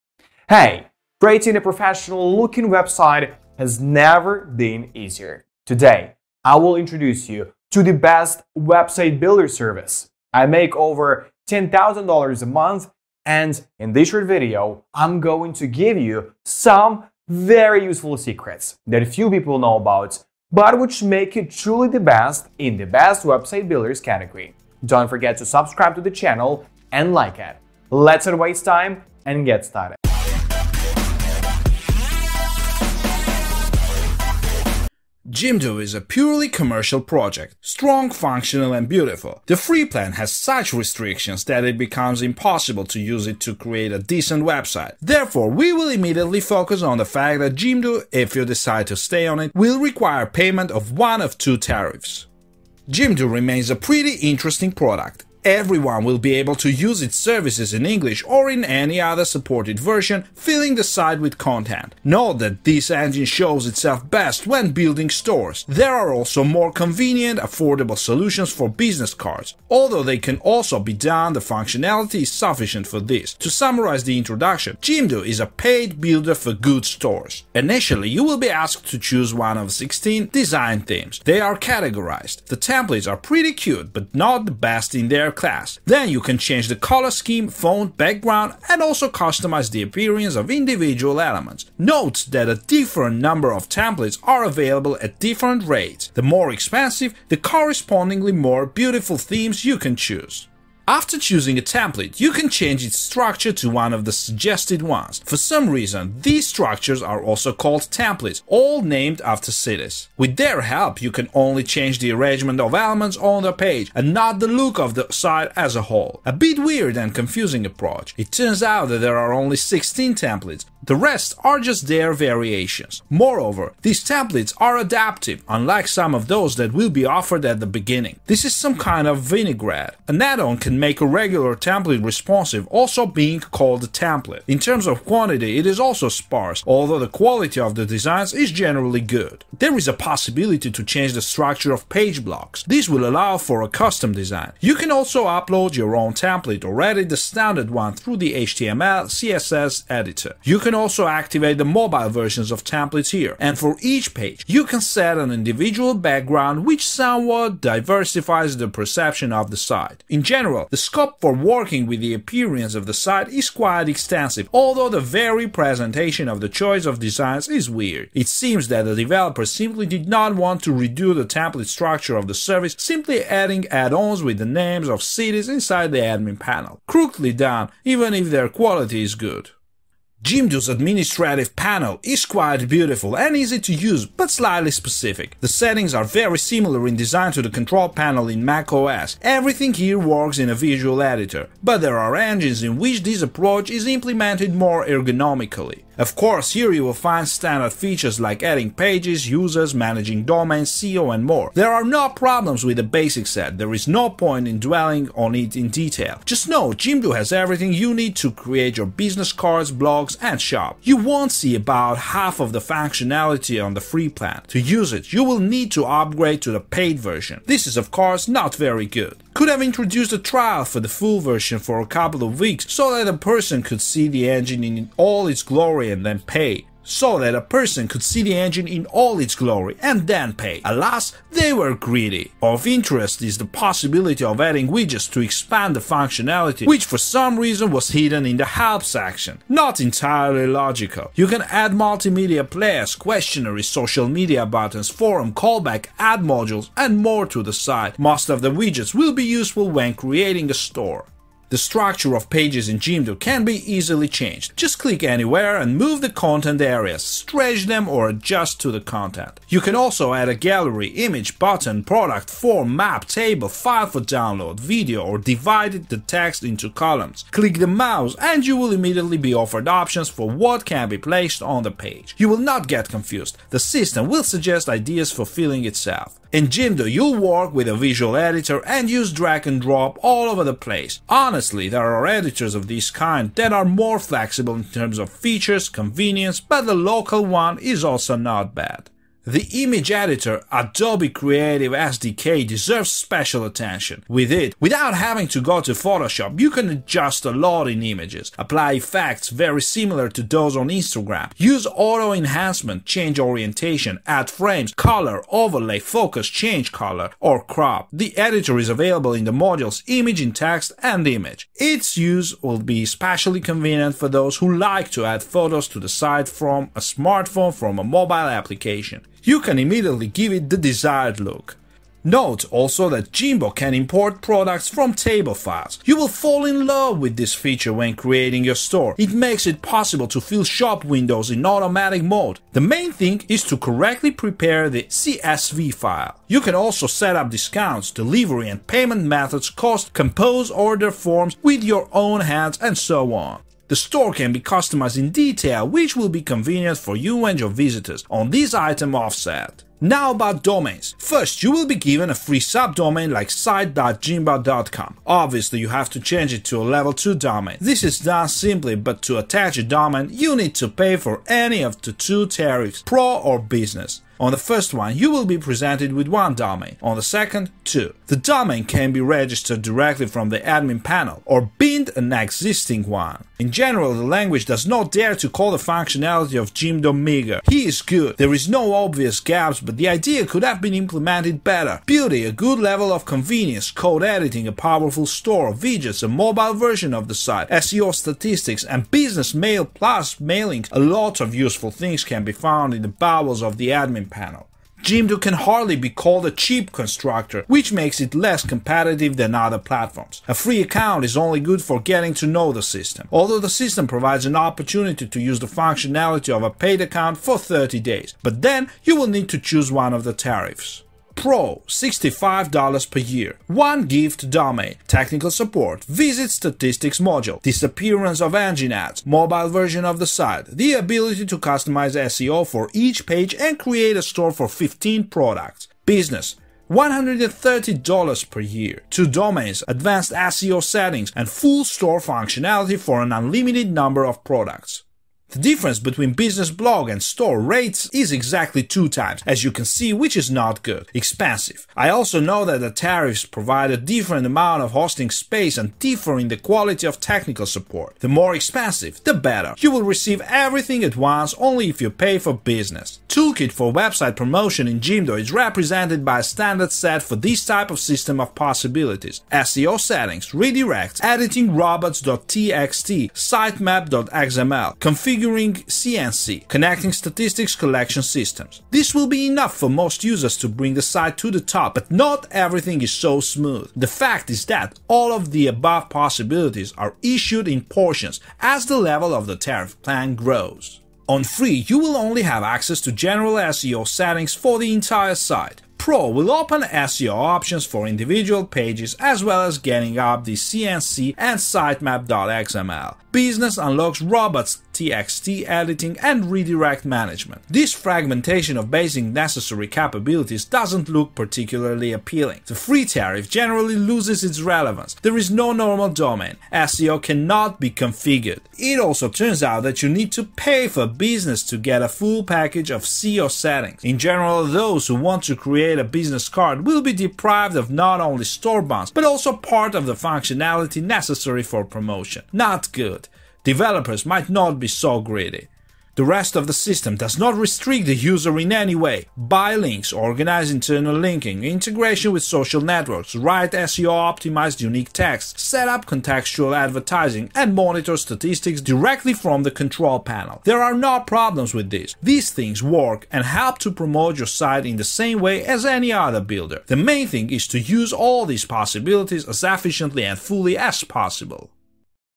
Best Website Builder For Business Review 2021🔥
Hello, today I'm going to tell you about the best Website Builders service.  I have been making over $ 15,000 a month using this service and have been using it for seven years now.  It is a very versatile and highly efficient assistant.  In this short video, I’ll reveal to you five very useful secrets that few people know about, but which make it truly the best in the Website Builders category.  It's about Jimdo, I'll leave the link to the official website under the video.  Let's not waste time and get started!